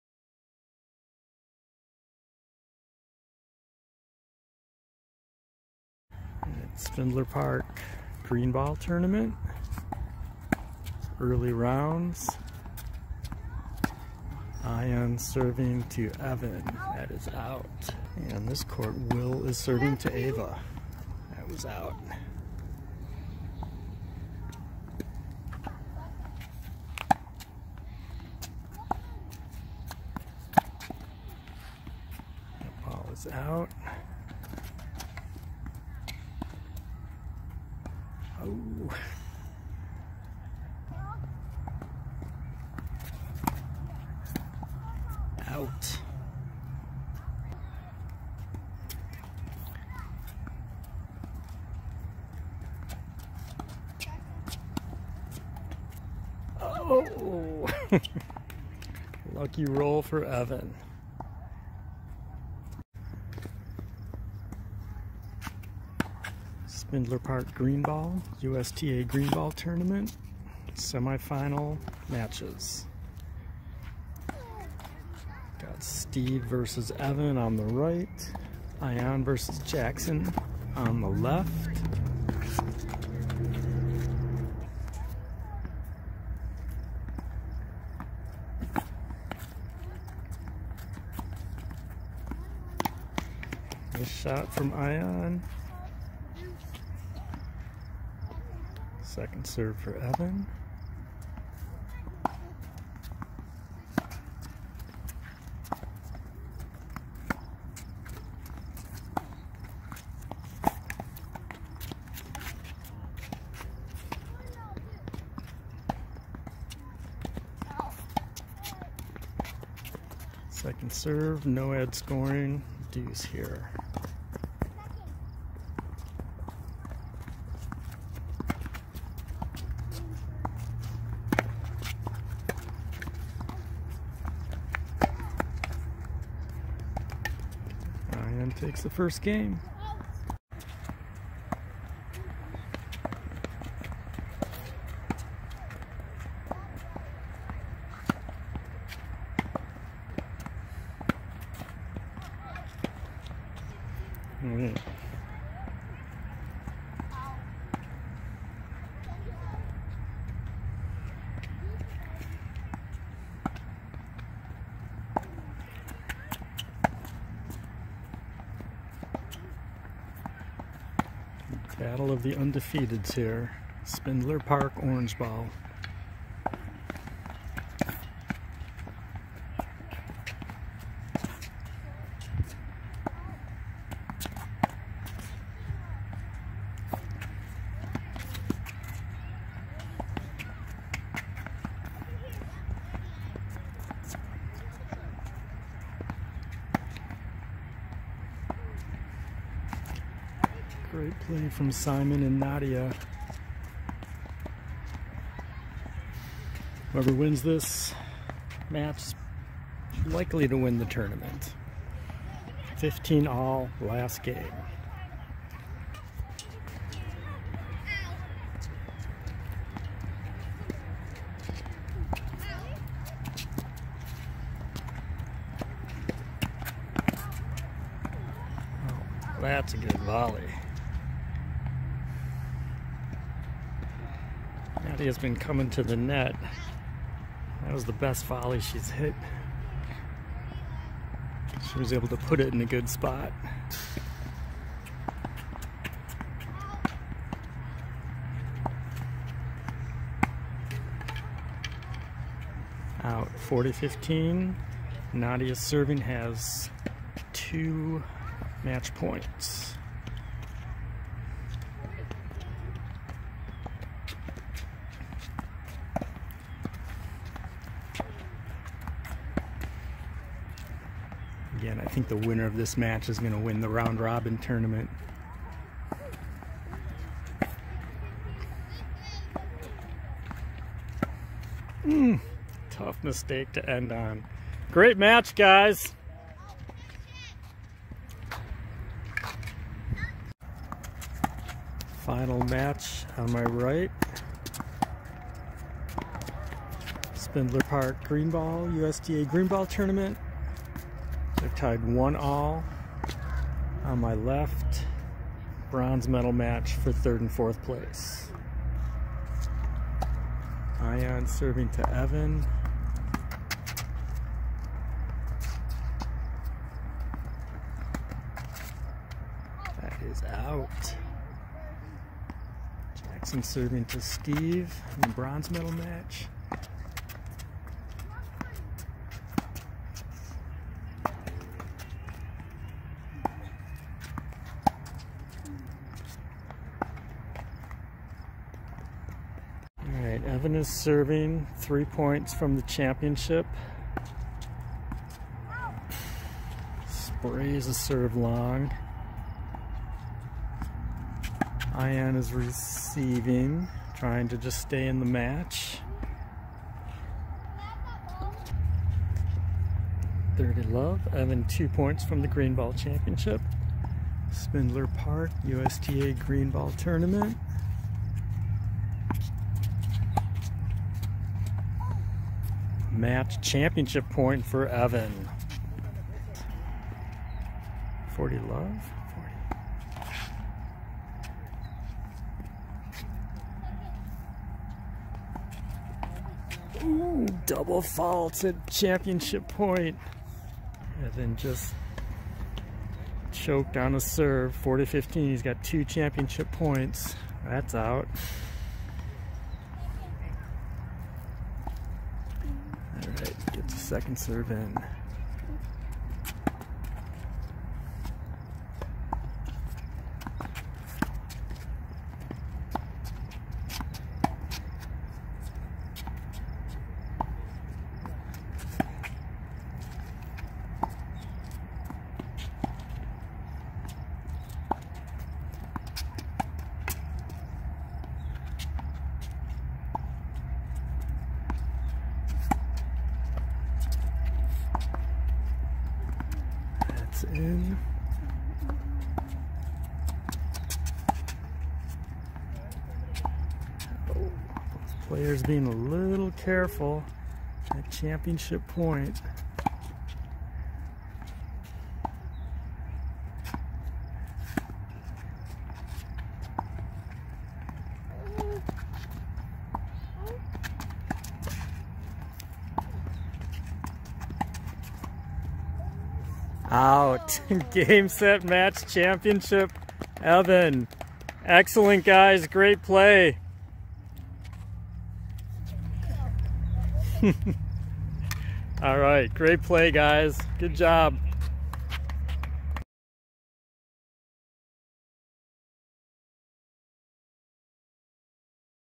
Spindler Park Tennis Tournament, USTA Orange & Green Ball
10 years and under tournament at Spindler Park, Eastpointe MI, Sept 2020. Tournament director, Chip Fazio, ifirstserve.net.